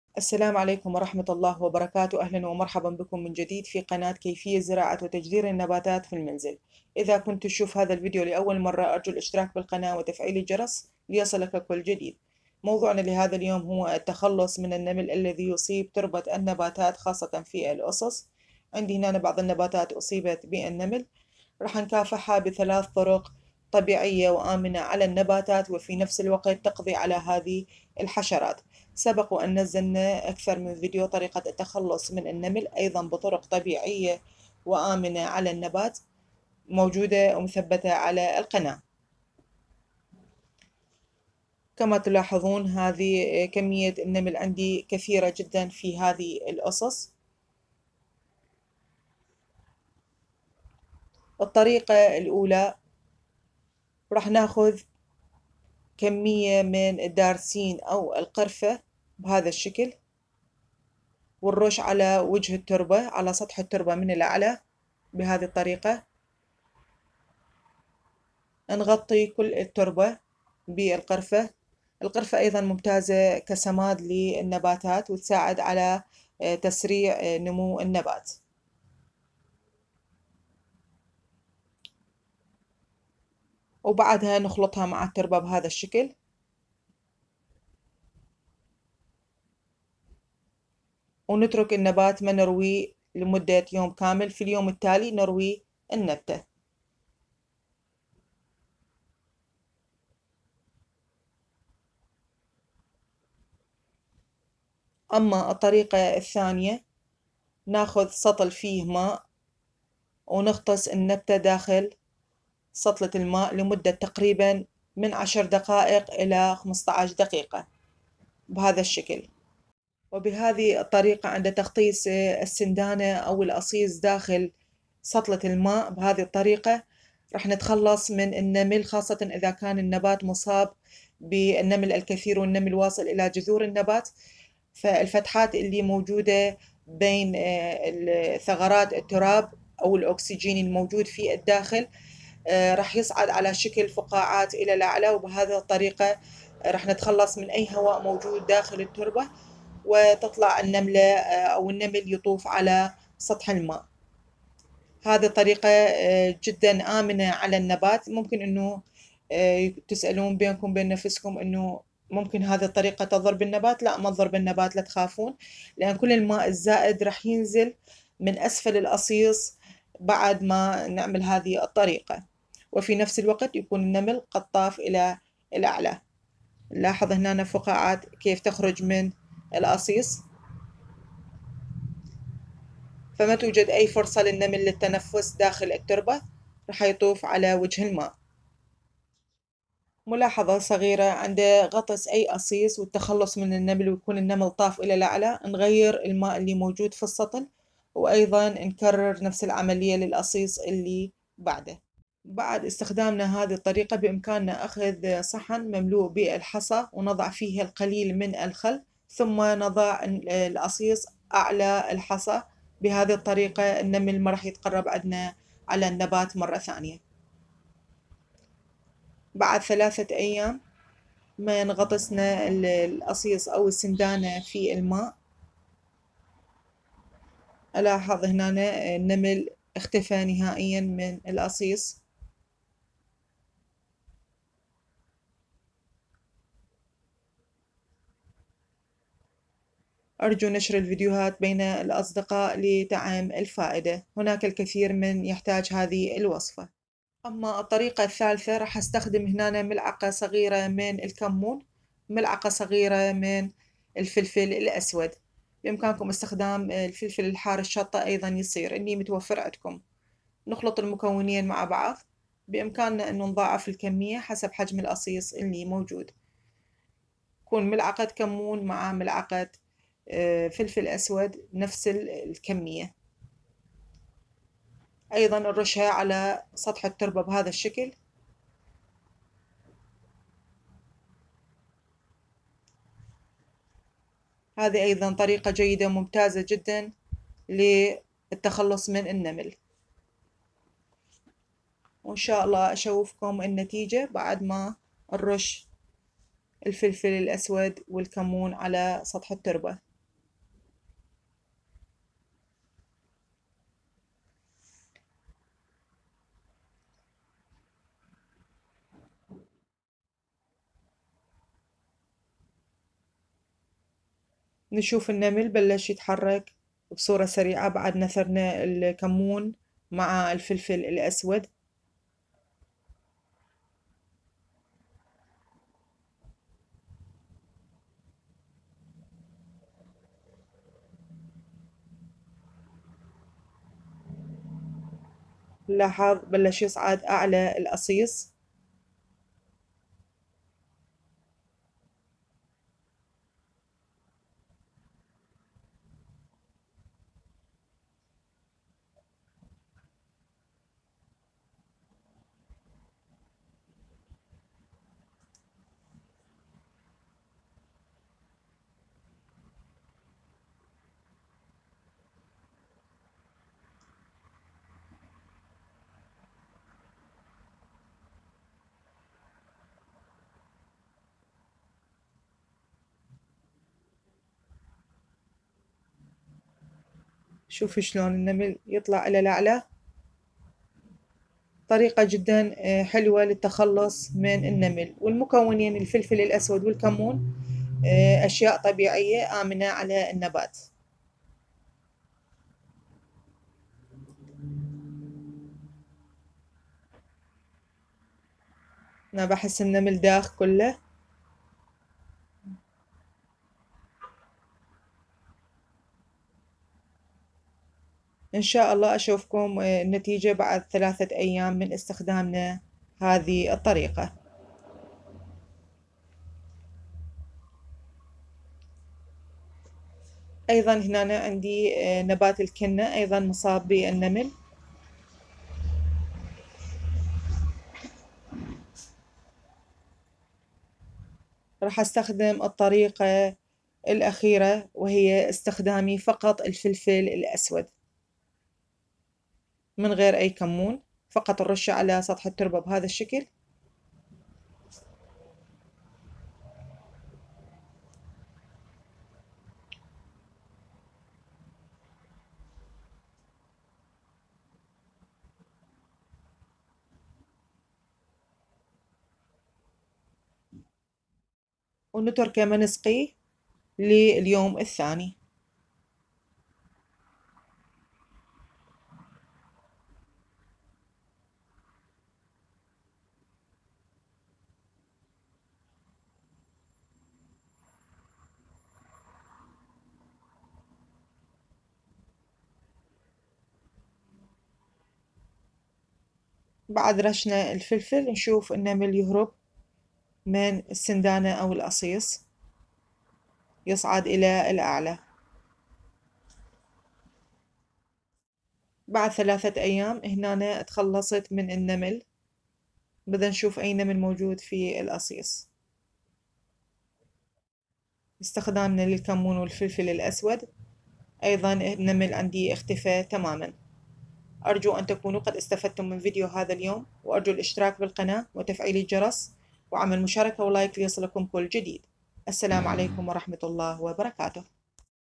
وصفة سحرية للقضاء على مشاكل النباتات والتخلص من النمل داخل الاصيص بثلاث طرق قوية وآمنة وفعالة
السلام عليكم,
أهلا وسهلا بكم في قناة كيفية زراعة وتجذير النباتات في المنزل HowTo Plant
للاشتراك في القناة اضغط على هذا الرابط